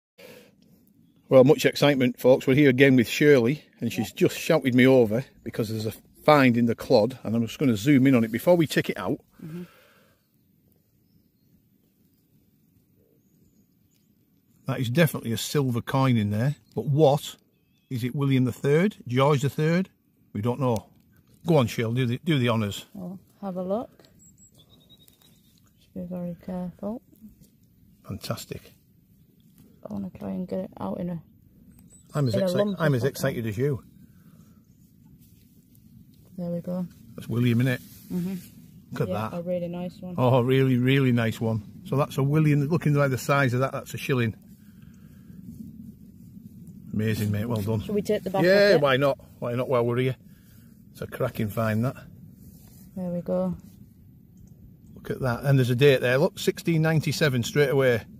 Amazing silver coin footage!
Amazing silver coin footage directly from the field! Metal detecting surprise live from the field, a fantastic glimpse of a silver coin being unearthed on the unearthed organised dig, full film to see is
Not often you see coins of this age come up live from the field!